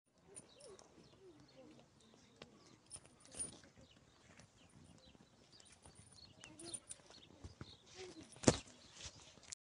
سلالة الابقار الهولشتاين ابقار المانيا #2024
من افضل عشرة 🔟 انواع الإبقاء في العالم هي الهولشتاين